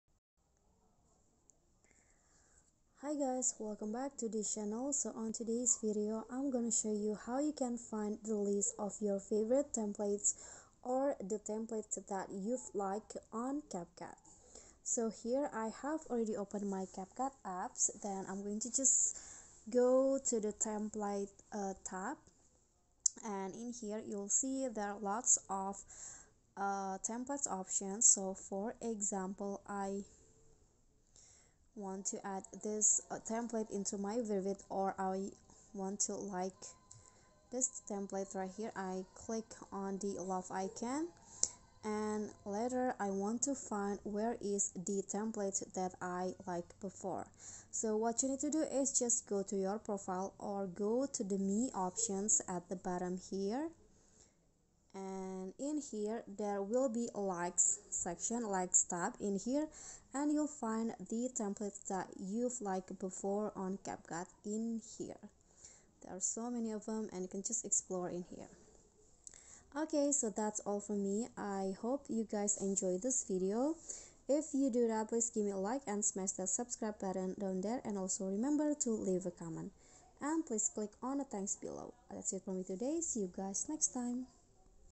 How Can You Find the List of Your Favorite Templates (Templates You've Liked) on CapCut?
Here's a way to find the list of your favorite templates on CapCut